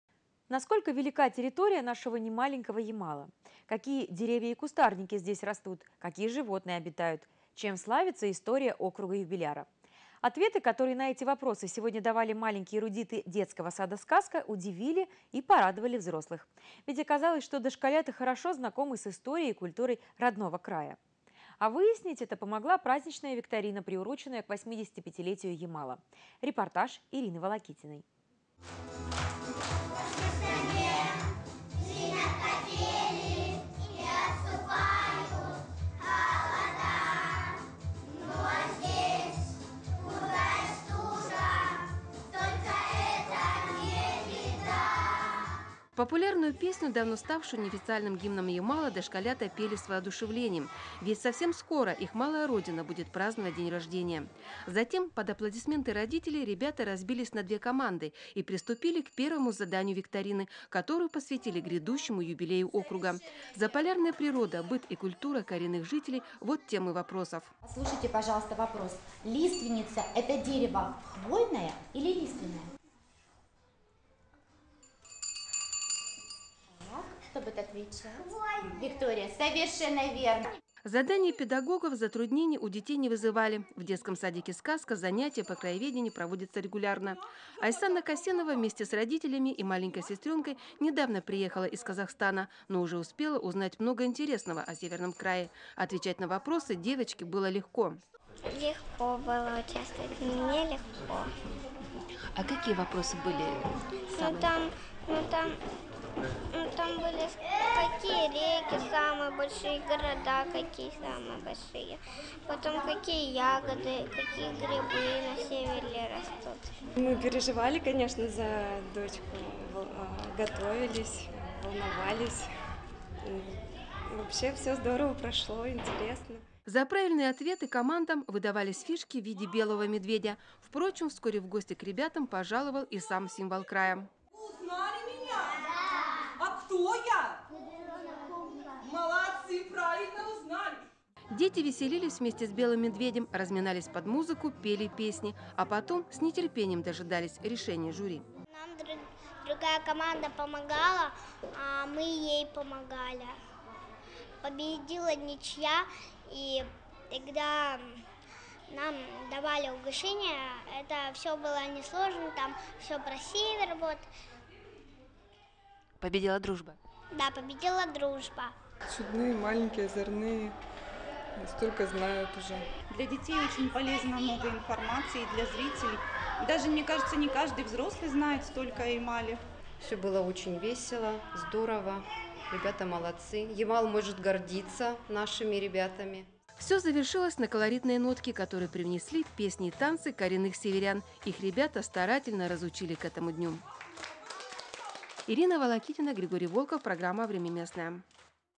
Наш немаленький Ямал
Воспитанники «Сказки» доказали, что они знают историю родного края на «отлично».  К 85-летию округа в детском саду приурочили праздничную викторину.